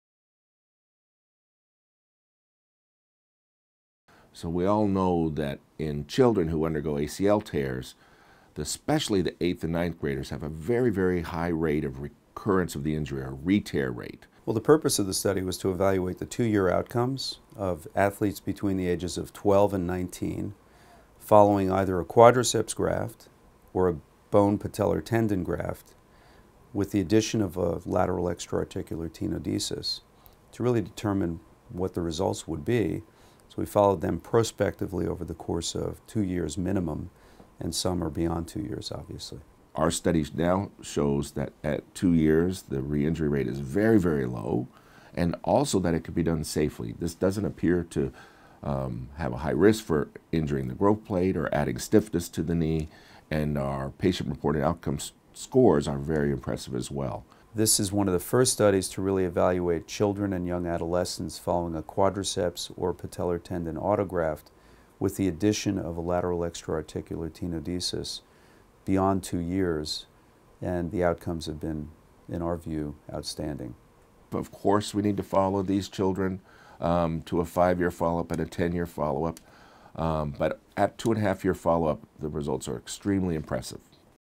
HSS Minute: Adolescent ACL Reconstruction Clinical Outcomes Study
A new study by researchers at HSS presented at the @AmerAcadOrthoSurg (AAOS) 2022 Annual Meeting found that a novel surgical technique performed during anterior cruciate ligament (ACL) reconstruction in certain younger patients garnered positive results two years post-surgery. The study evaluated the safety and efficacy of performing a lateral extra-articular tenodesis (LET) using a modified Lemaire technique (MLT) in conjunction with an ACL reconstruction in children and adolescents who are at an increased risk for a failed ACL reconstruction. Learn more from Drs. Daniel Green (senior author and pediatric orthopedic surgeon) and Frank Cordasco (lead author and sports medicine surgeon)

"HSS Minute" is a video series geared towards educating patients on the expertise and innovations at HSS.